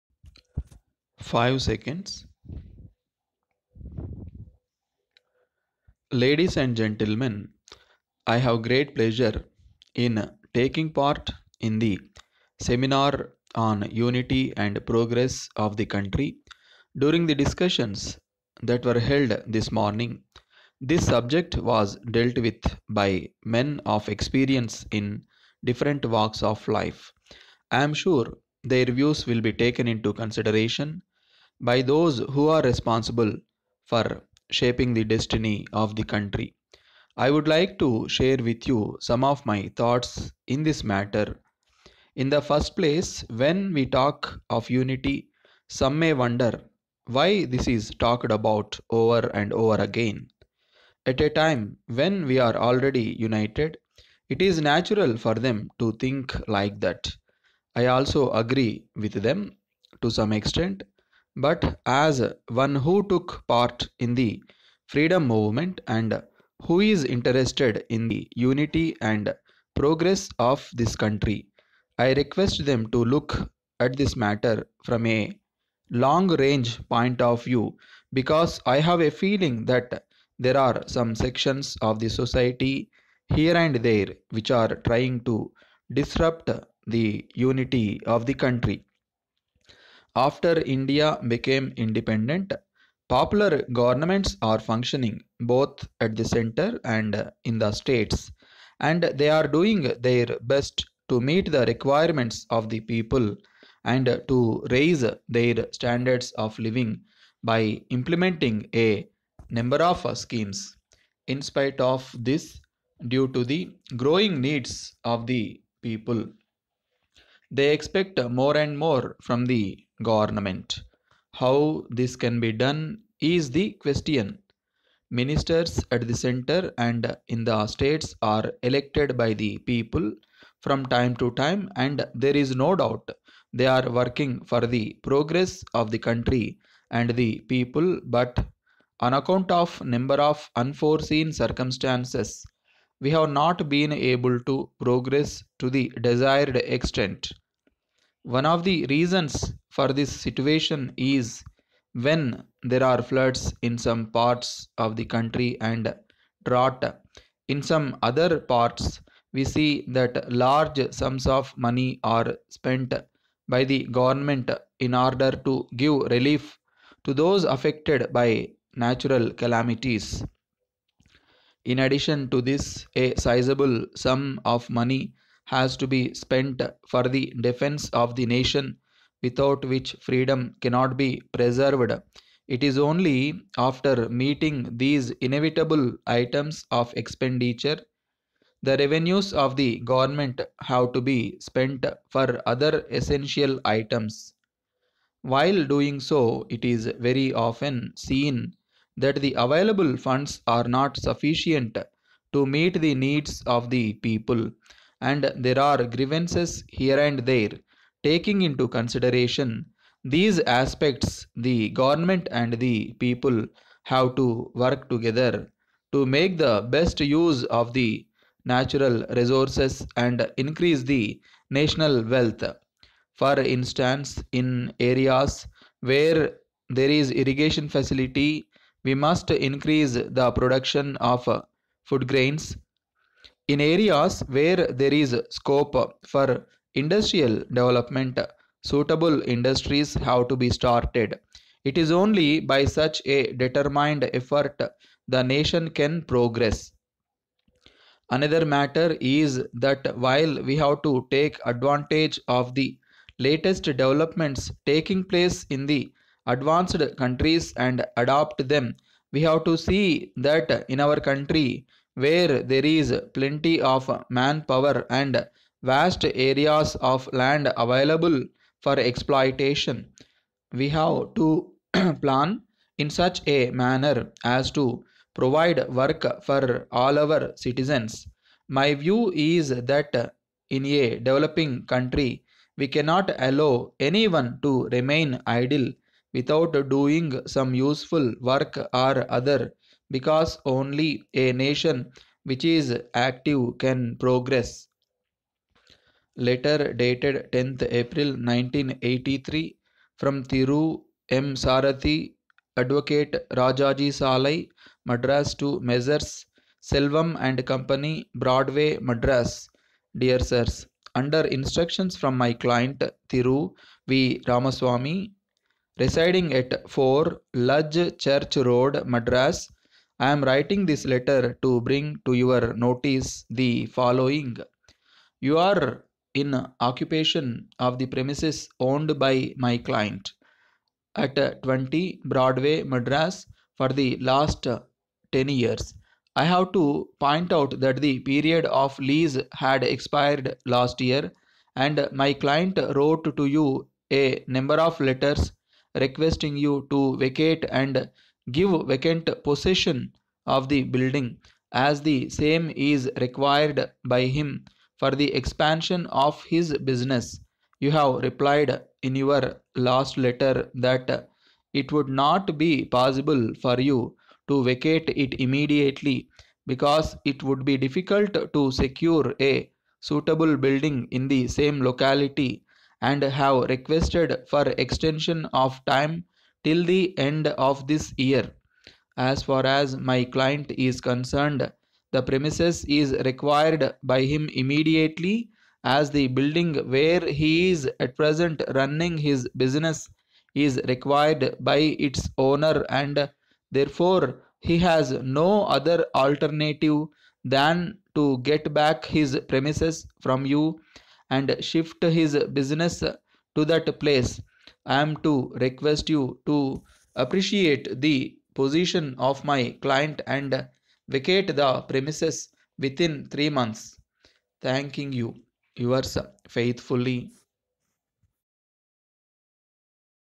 TAMILNADU STATE's SHORTHAND ENGLISH DICTATION @ 120 WPM OF JUNE, 1985, SENIOR GRADE, BOOK CODE-G4
120 WPM, SHORTHAND ENGLISH DICTATION OF TAMILNADU STATE OF JUNE, 1985, SENIOR GRADE, BOOK CODE-G-4